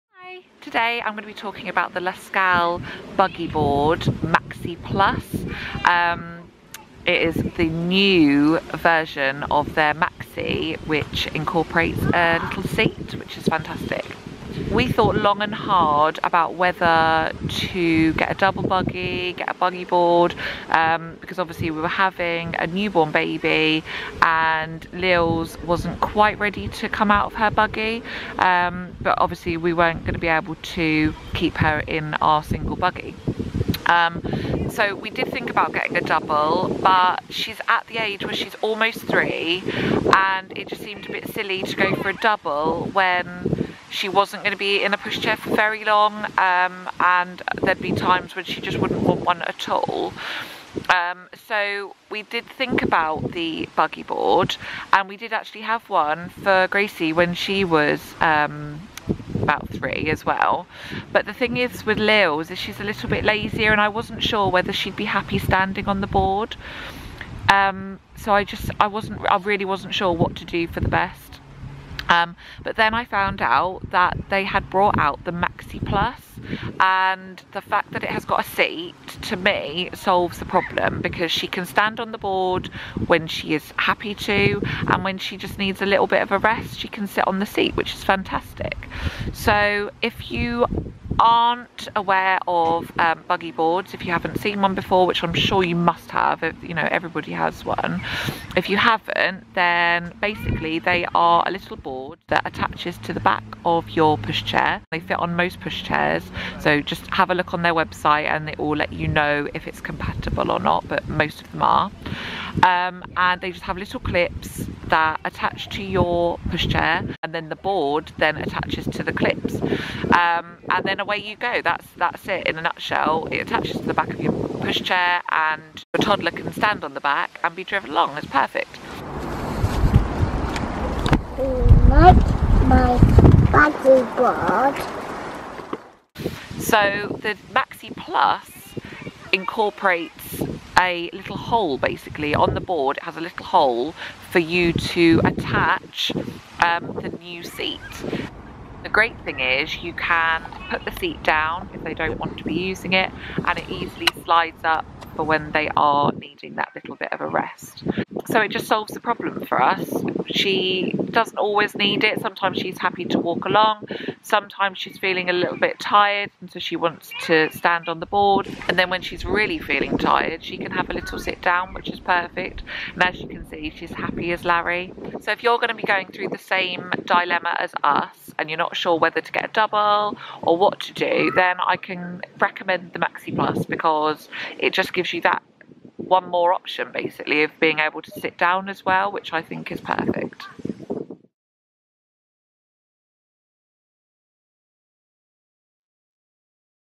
Buggy Board Maxi Plus
The Buggy Board Maxi Plus is the perfect solution when you have a toddler and a baby. The new seat gives you an extra option, allowing your little one to have a sit down when their legs get too tired to stand on the board.